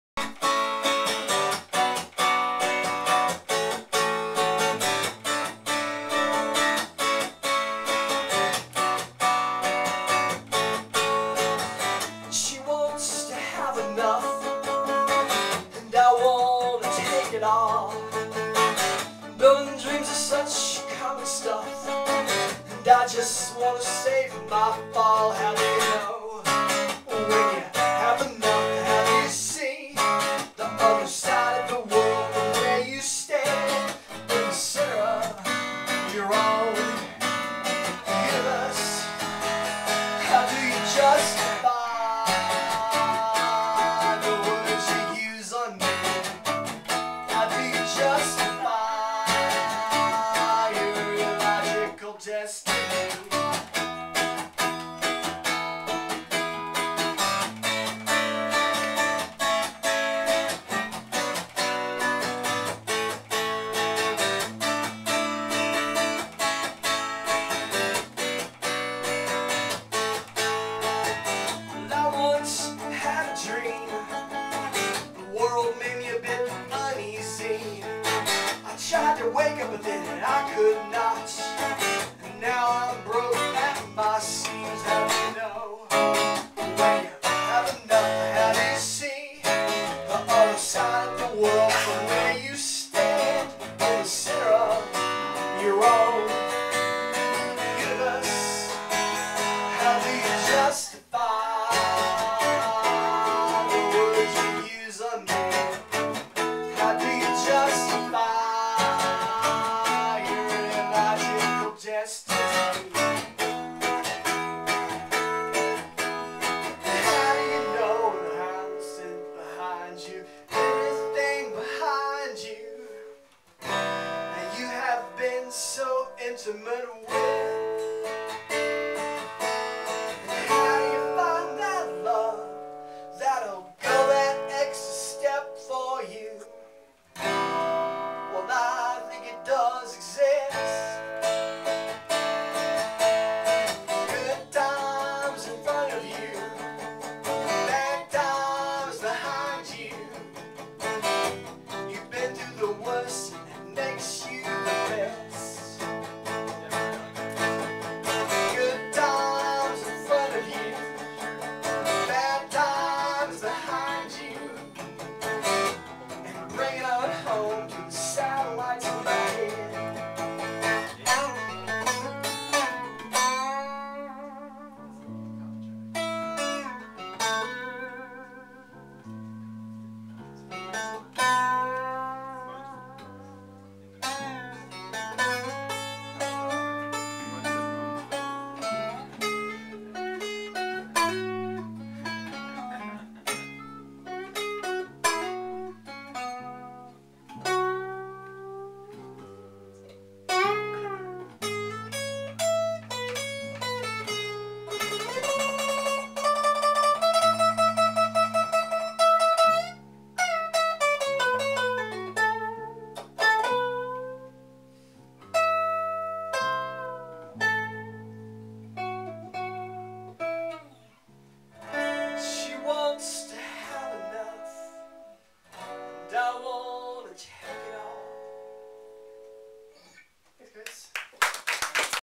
jane&scottshow4.avi
THe Jane and Scott Show live at Bread December 2 2009